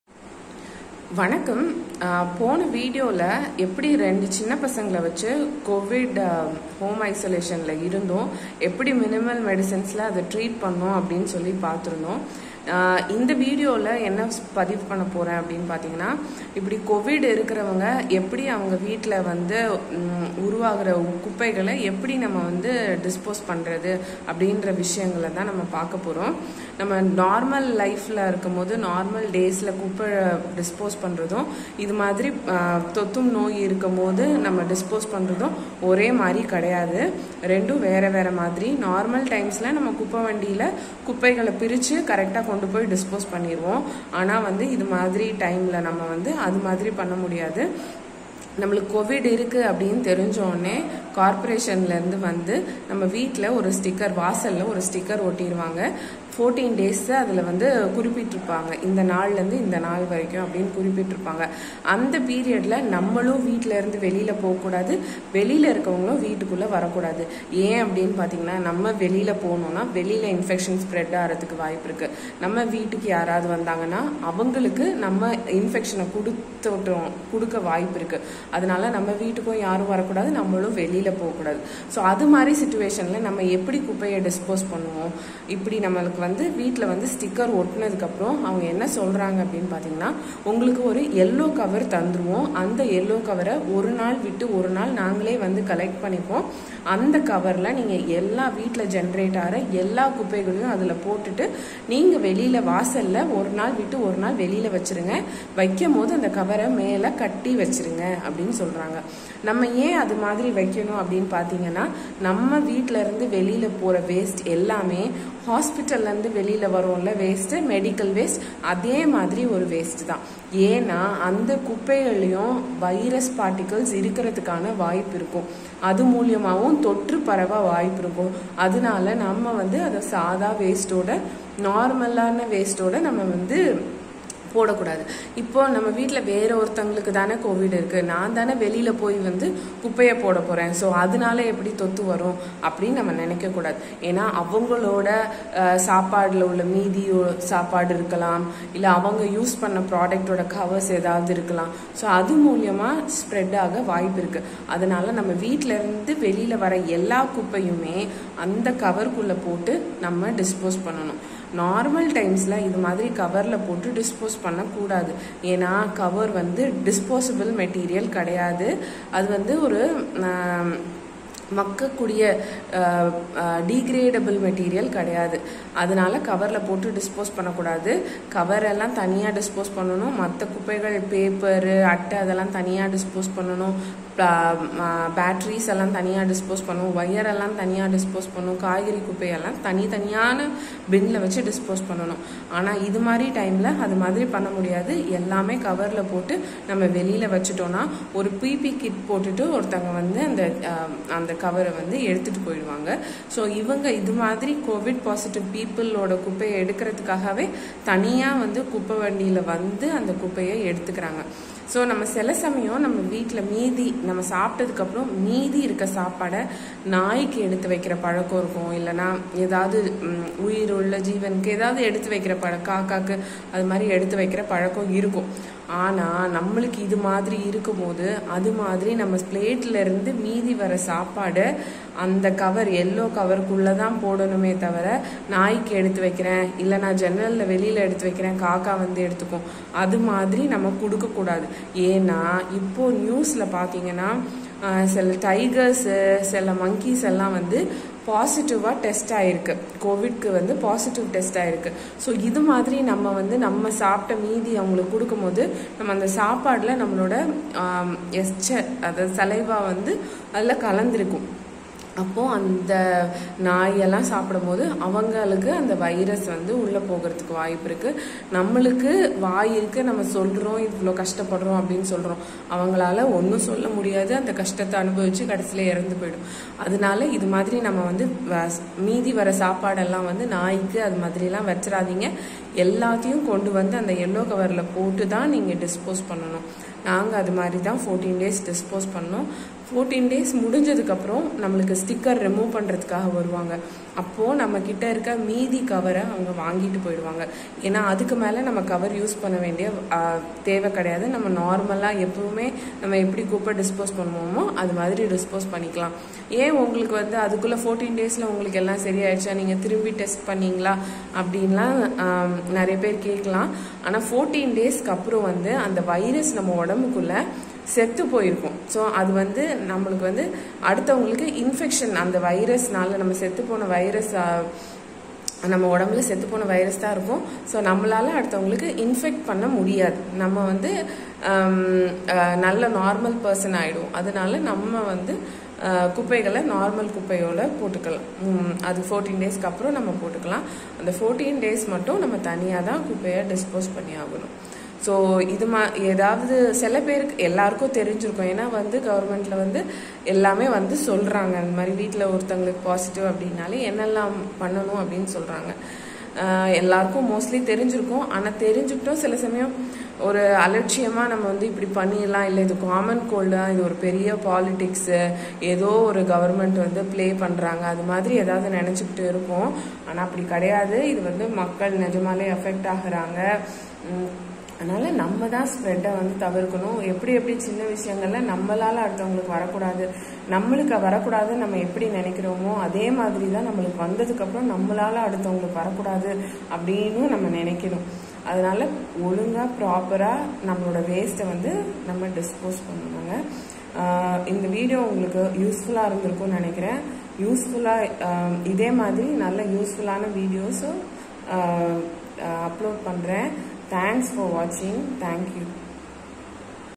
How to dispose waste during CoVID +ve time| Proper way to dispose waste during Home Isolation
Waste generated from a CoVID +ve house is not a normal waste, its a medical waste & it was be disposed properly in a specific way. Do watch the video to know how to dispose it properly and share with others to create awareness.

Thanks 💖 @AbhinayasMenuBook